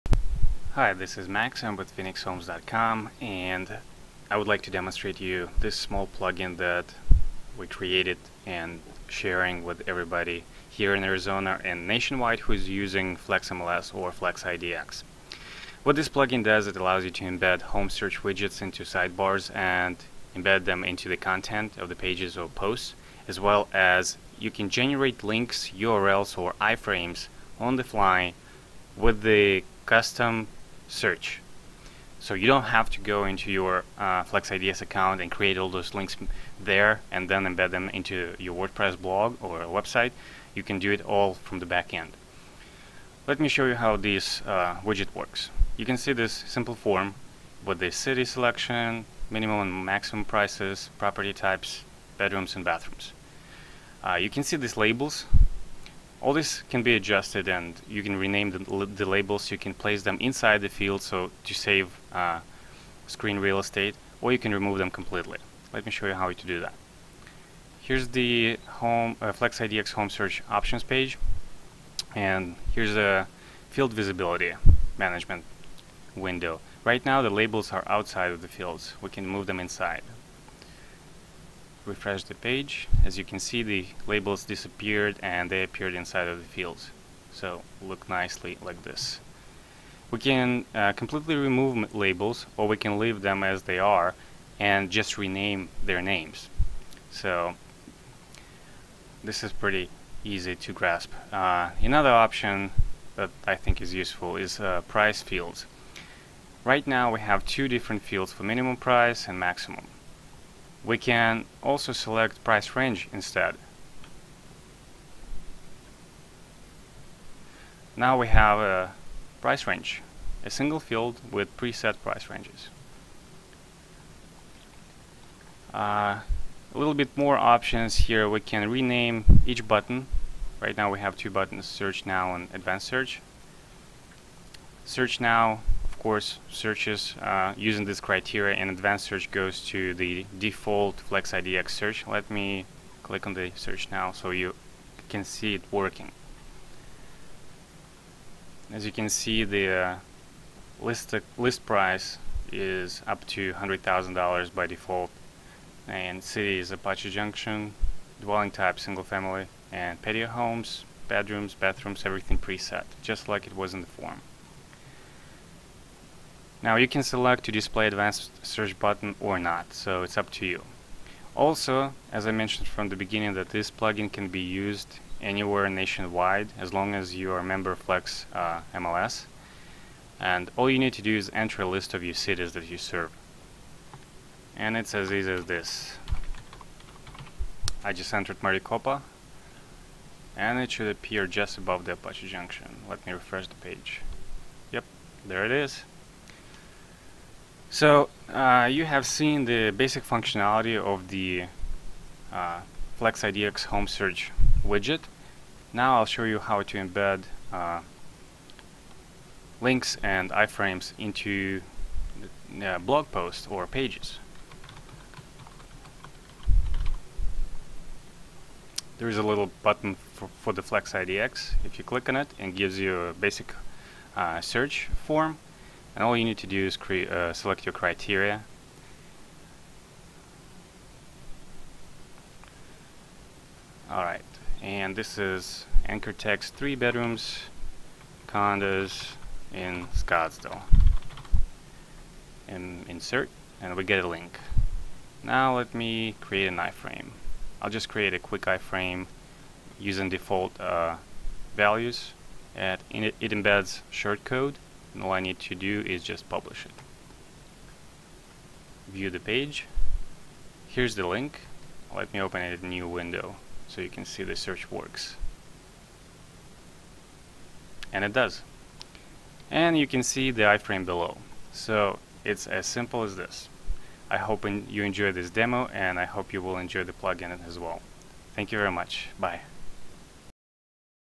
flexIDX Home Search plugin for WordPress
flexIDX Home Search plugin provides an easy solution to embed home search form into your sidebars, and page/post content. Generate custom search links and iframes withing WYSIWYG editor and embed them into your community related pages with a click of a mouse.